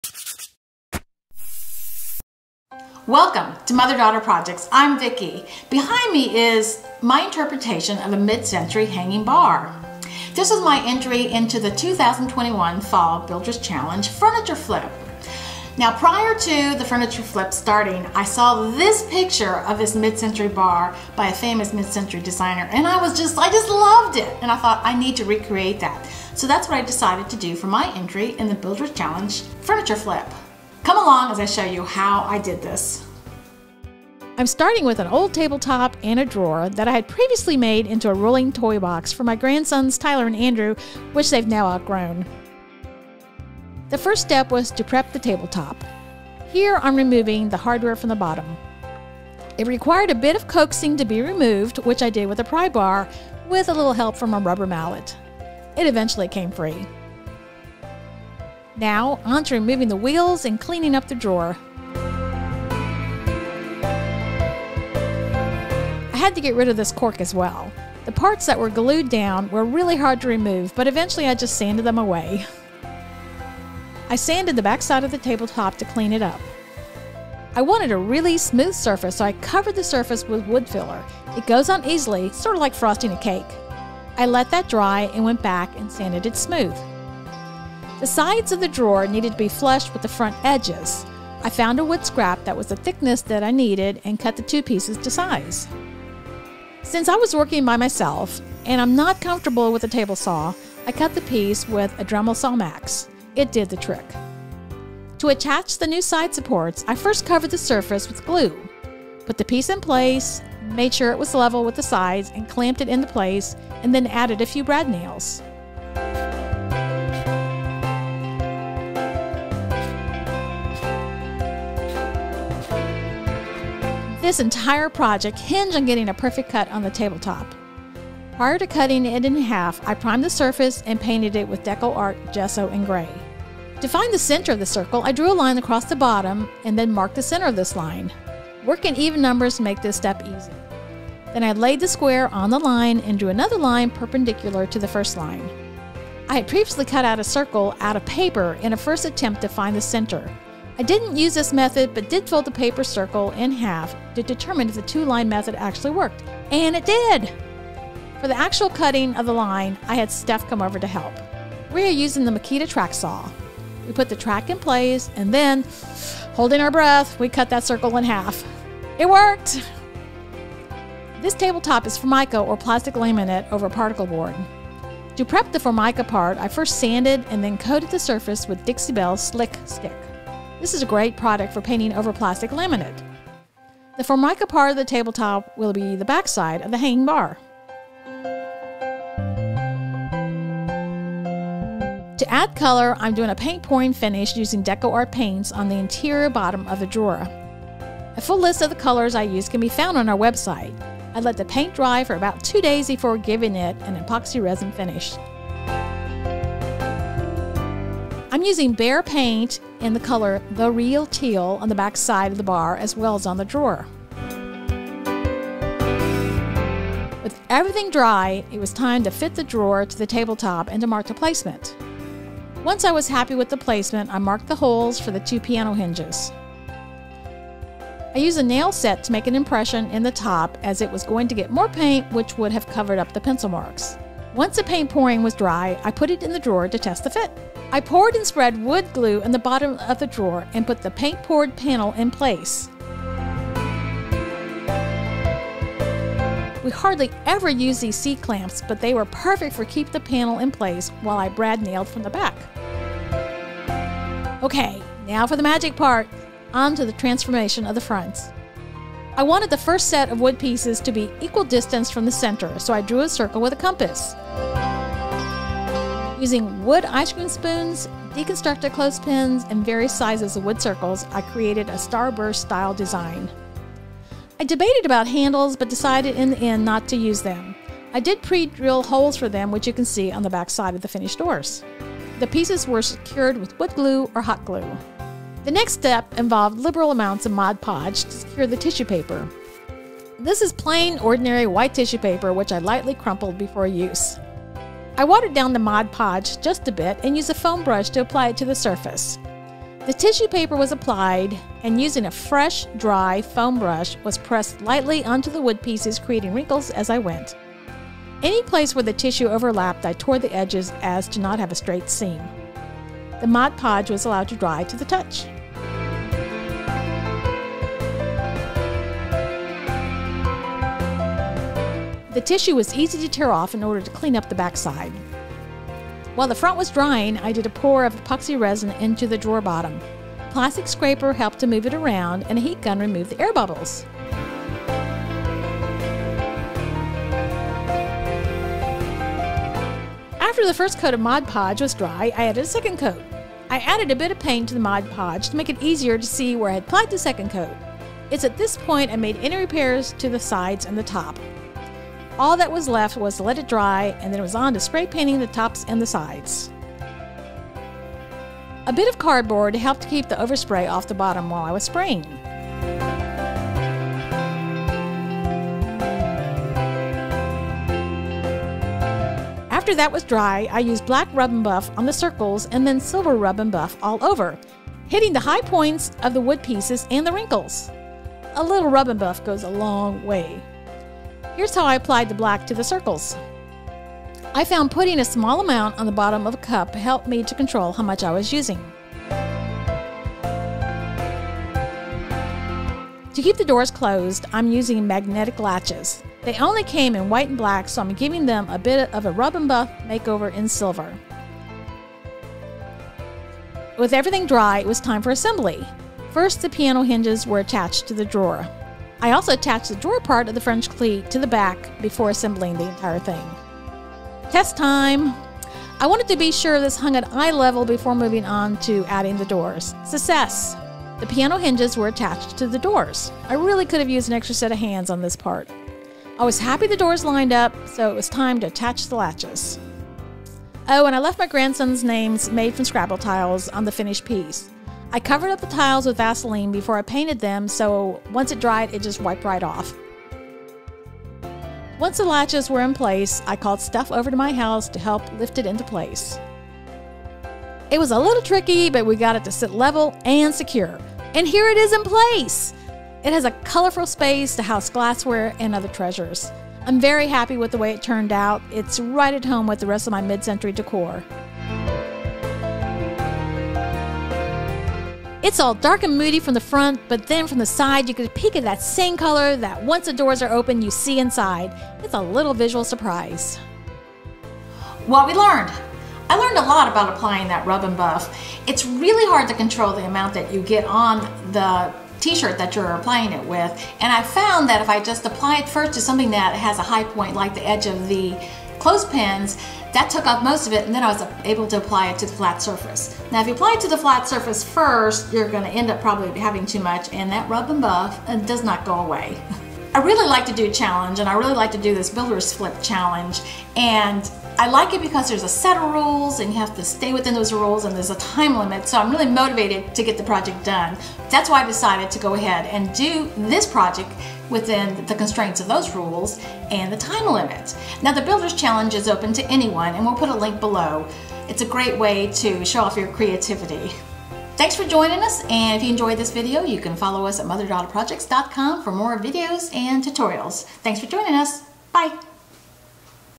How to Make a Mid-Century Modern Hanging Bar
Join Vicki as she tries to make a DIY version of a furniture piece by her favorite mid-century designer Adrian Pearsall!

This video is professionally closed captioned.

Steph wrote her memoir.